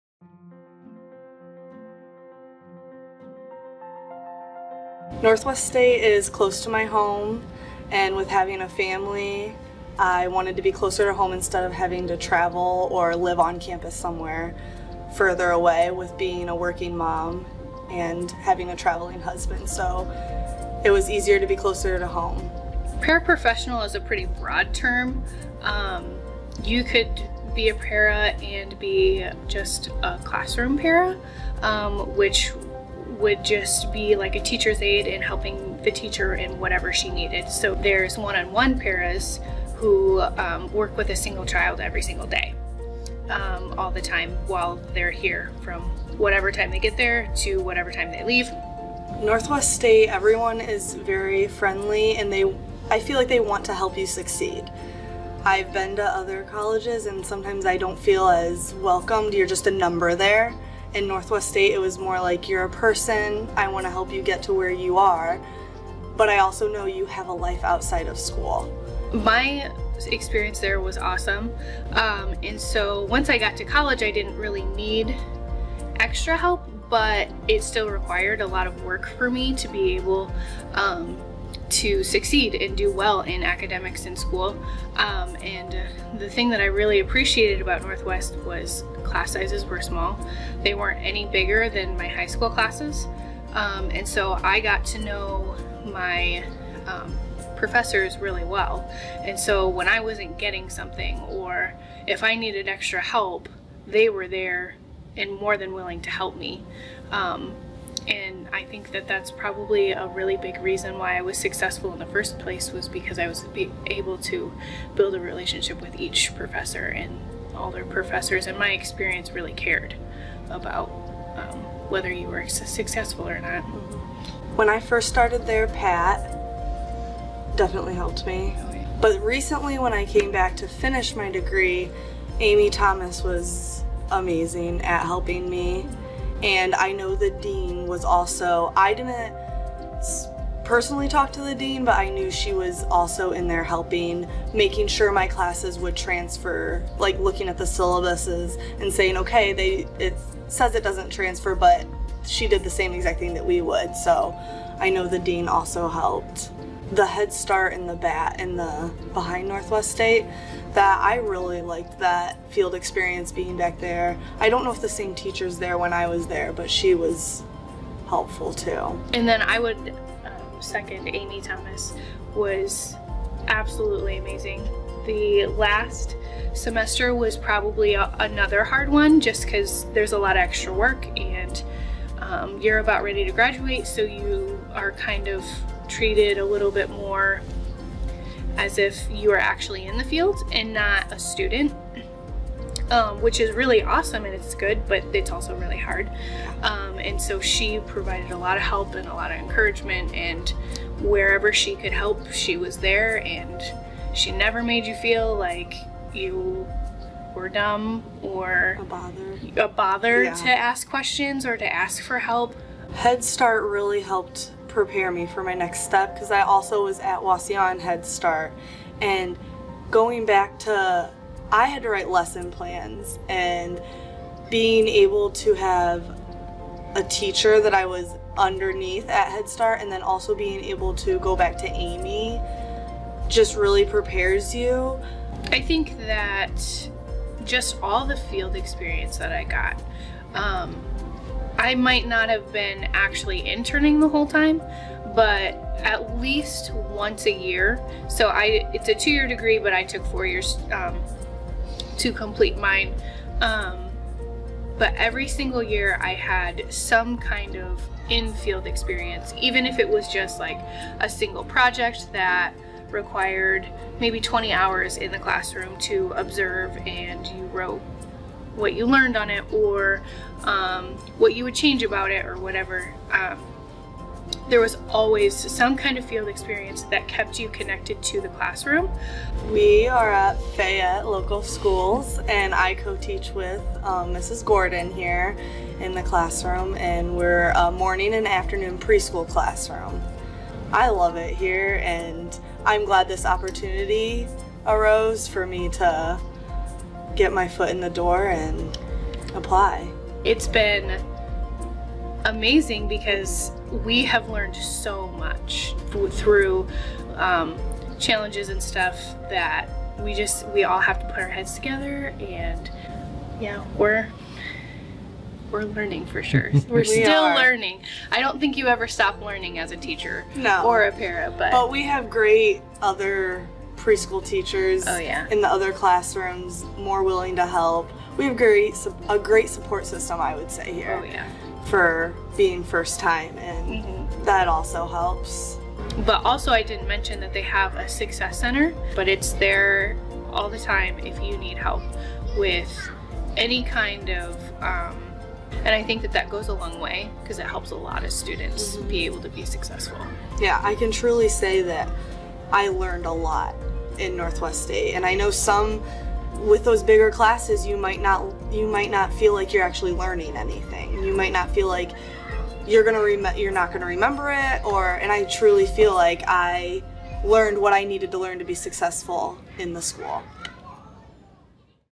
NSCC Proud: Erin Ford & Paige Grosjean
Erin Ford majored in paraprofessional and Paige Grosjean majored in pre-kindergarten at NSCC. They graduated together in spring 2019 and now share the same classroom in Fayette, Ohio. Both graduates shared their stories of success, including how their NSCC experience and education helped prepare them for a fulfilling career in the fields they studied.

Learn more about all of our programs at NorthwestState.edu.